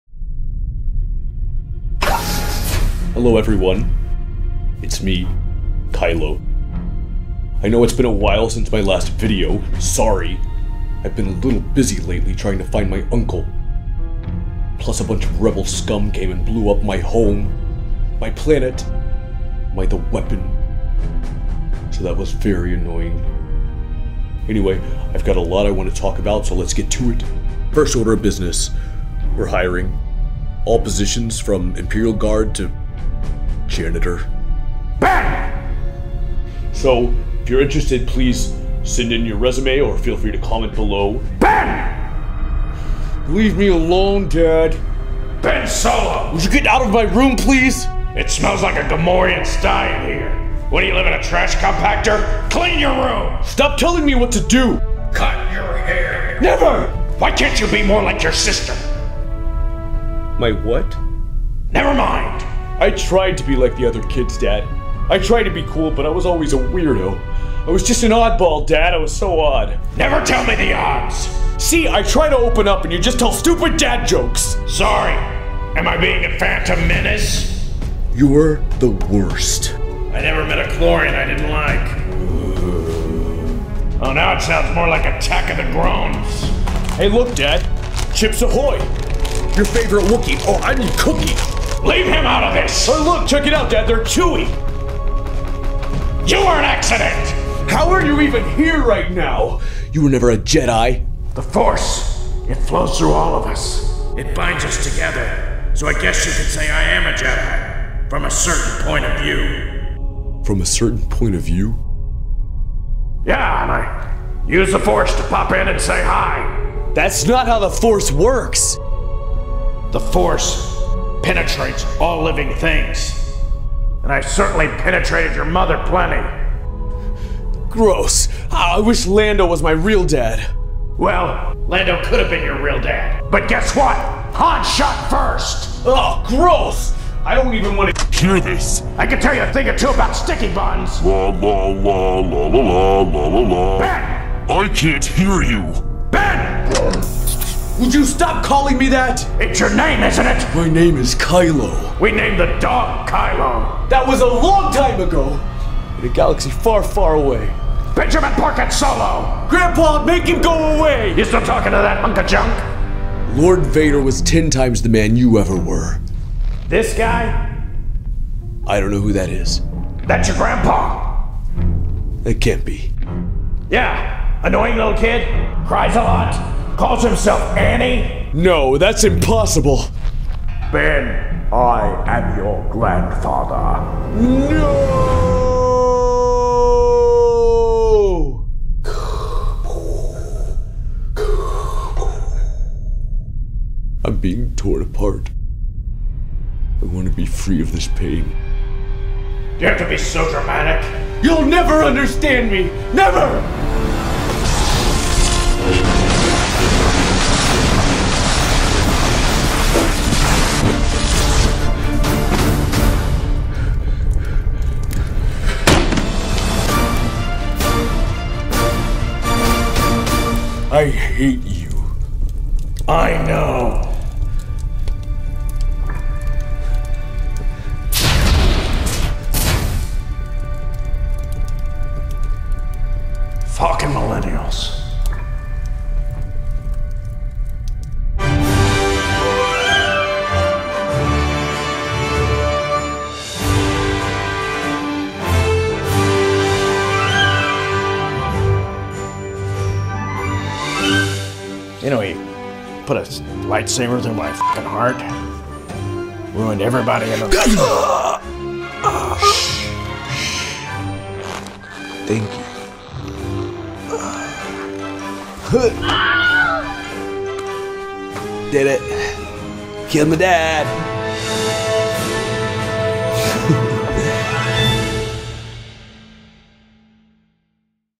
Kylo & Han: A Star Wars Story
Leaked footage from Star Wars Episode 8. Kylo Ren regroups after Star Killer Base is destroyed, but gets interference when trying to communicate with his followers.

Starring
Randy Wade Kelley as Kylo Ren
Phillip Wilburn as Han Solo

Randy Wade Kelley  on Twitter and Instagram @randsomnotes
Facebook and YouTube /randywadekelley
Phillip Wilburn on Twitter, Instagram, Facebook & Periscope @phillipwilburn